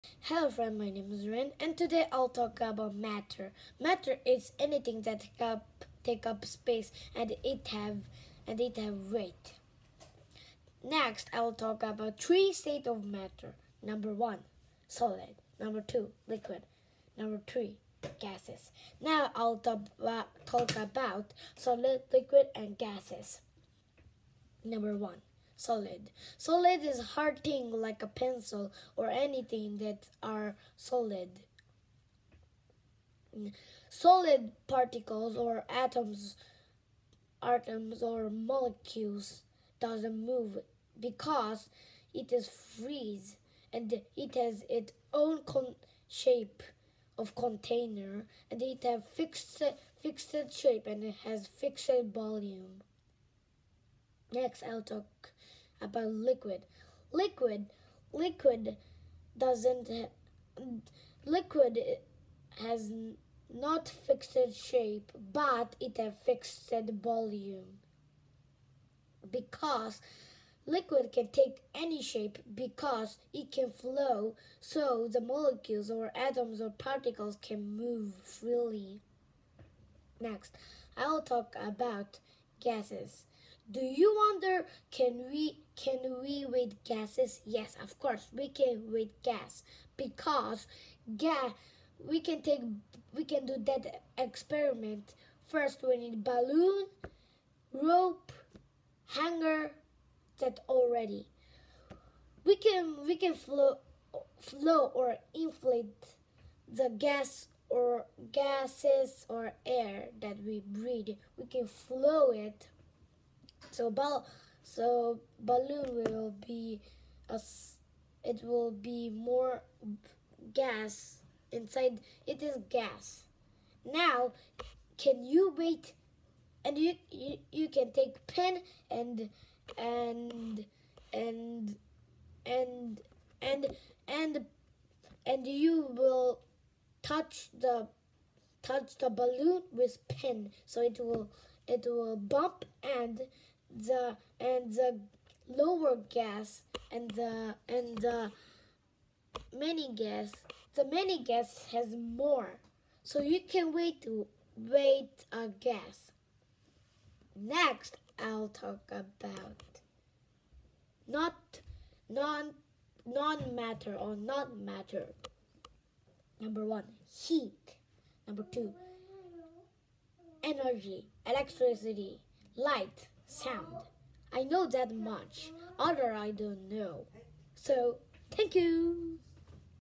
Talking about matter
I was talking about 3 state of matter example just little bit.